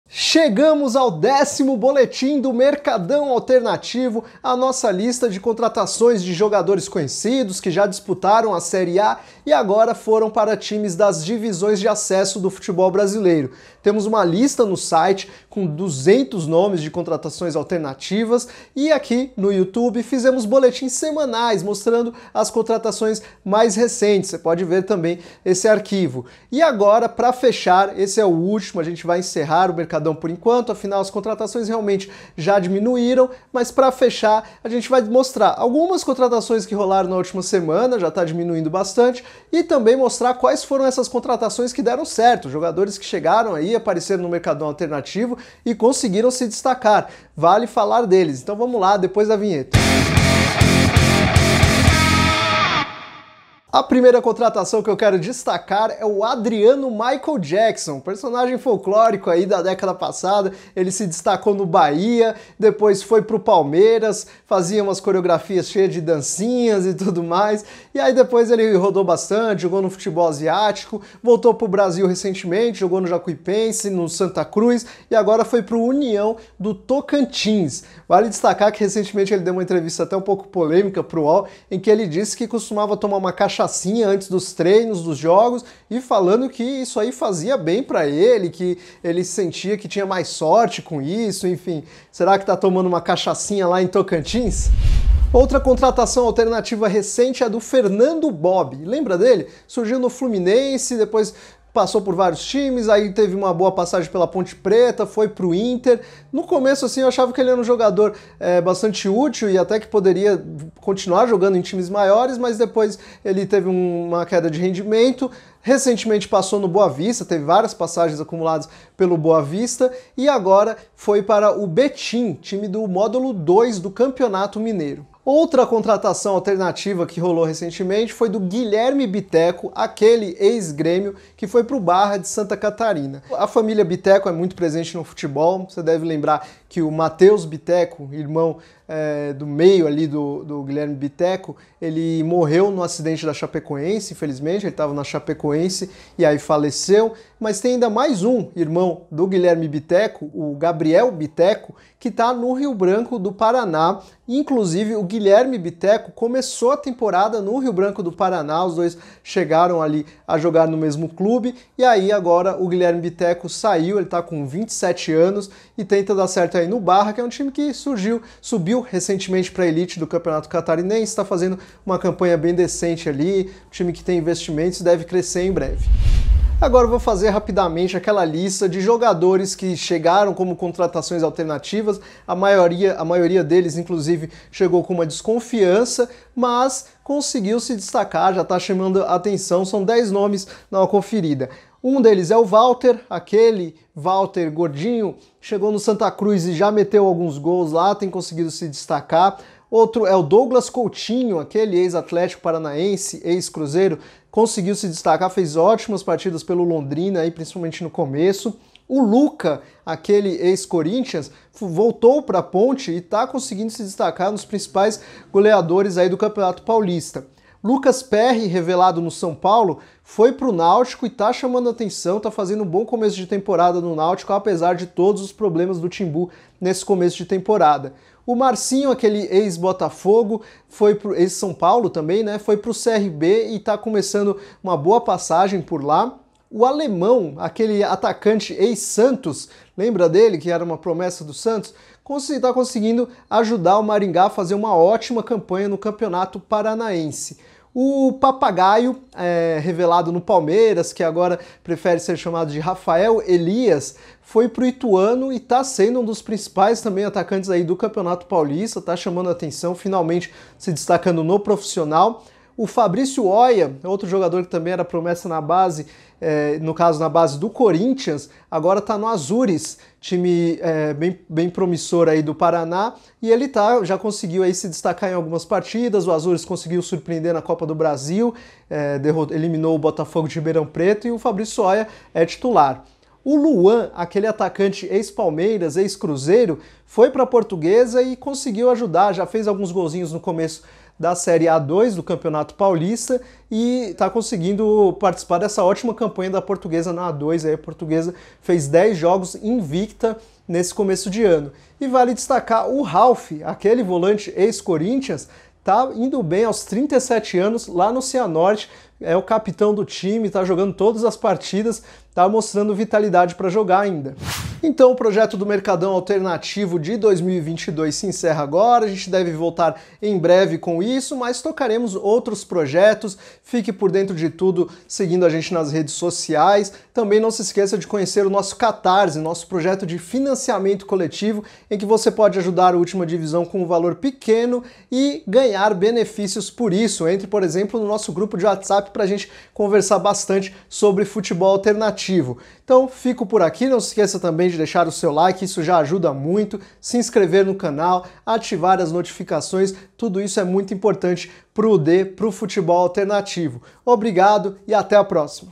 Boletim Mercadão Alternativo 2022 #10: Últimos nomes e as apostas que deram certo | UD LISTAS
Enfim chegamos à edição 10, a última do Boletim Mercadão Alternativo 2022. Como você já sabe, aqui contamos o paradeiro de jogadores como Adriano Michael Jackson (ex-Palmeiras), Fernando Bob (ex-Internacional) e Guilherme Biteco (ex-Grêmio).

E como extra fizemos uma lista de jogadores famosos anunciados em boletins passados e que já estão se destacando em clubes de divisões inferiores do Brasil.

Lembrando que no Mercadão Alternativo nós rastreamos as contratações mais curiosas da temporada, com destaque para atletas que jogaram nos principais times da Série A e que hoje estão disputando as divisões inferiores do Brasil.

Tem algum nome faltando? Então deixe nos comentários que ele pode entrar no próximo boletim. E não esqueça de dar o LIKE neste vídeo.